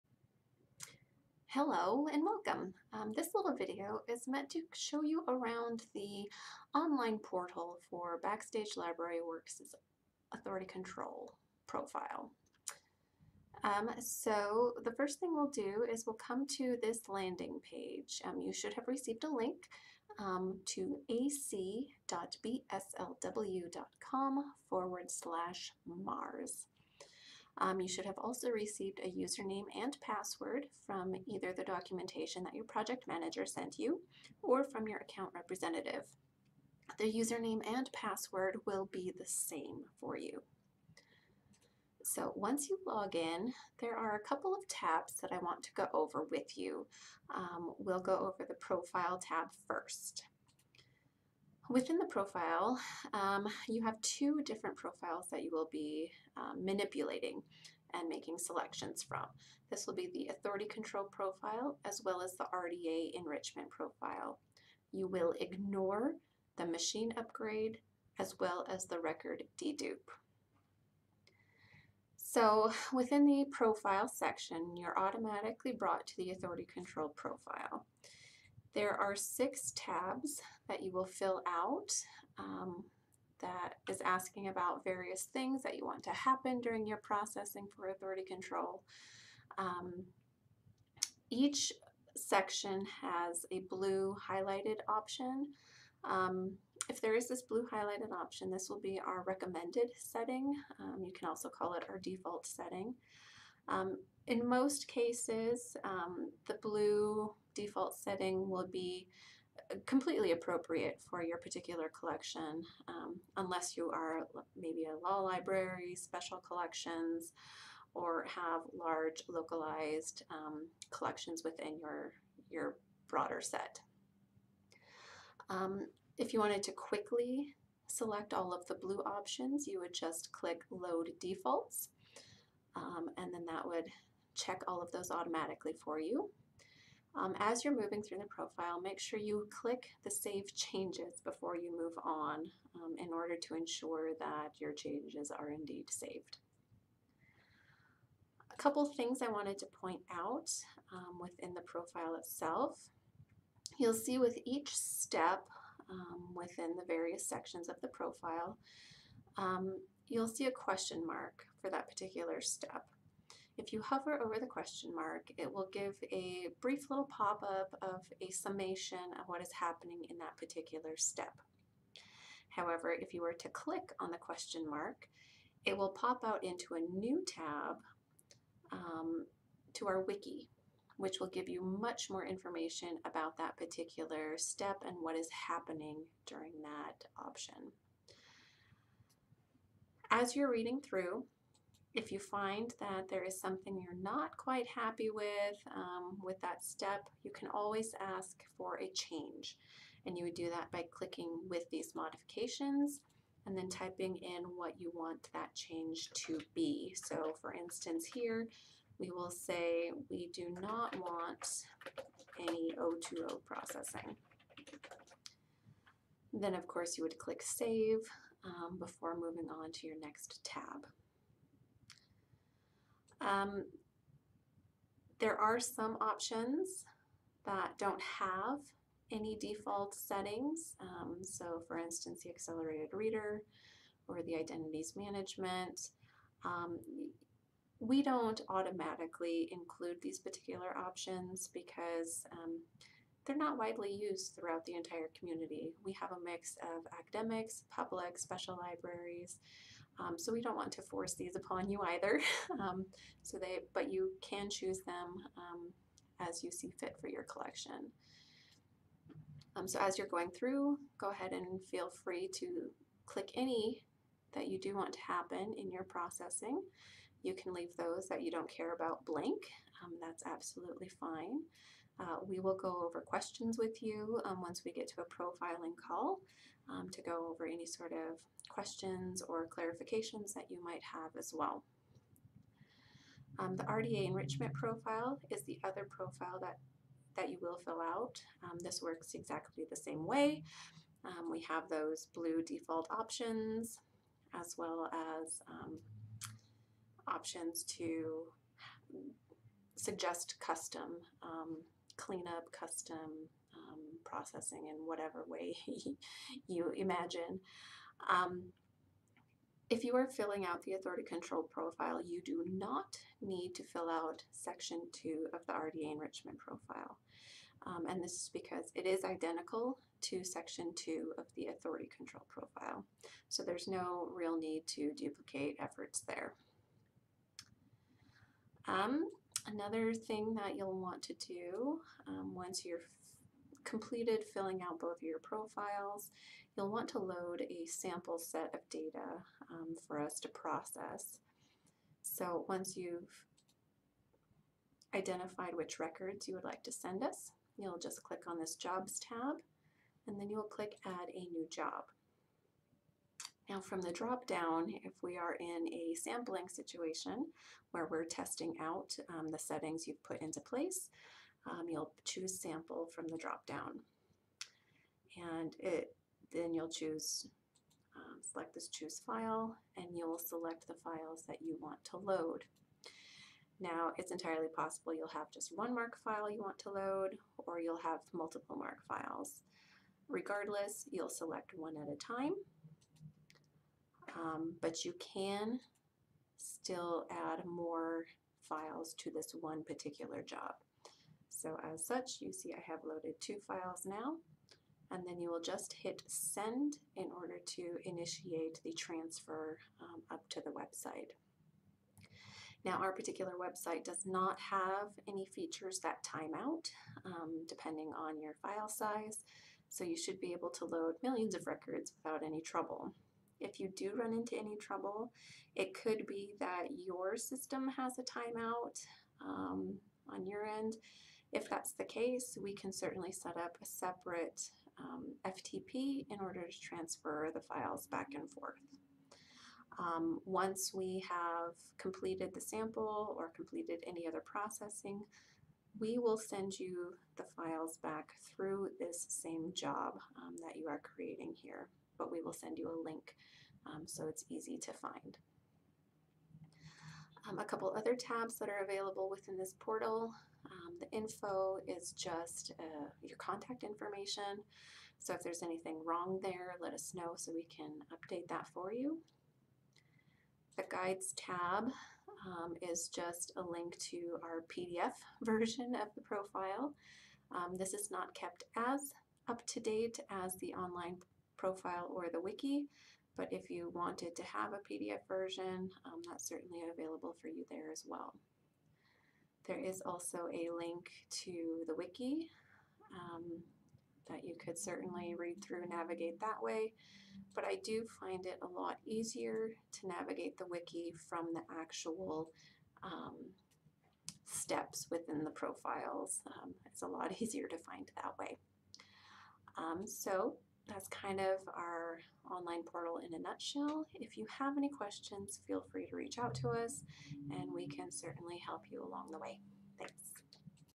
Navigating the Automation Portal
00:00 - Introduction
00:14 - Landing Page Walkthrough
00:42 - Profile Tab
01:12 - Authority Control Profile
04:43 - RDA Enrichment Profile
05:29 - Providing a Sample Set
07:26 - Info, Guides, Wiki Tabs

To learn more about how our processes interact with different library catalogs and which options you may want to consider, you can watch our webinars that focus on Authority Control in: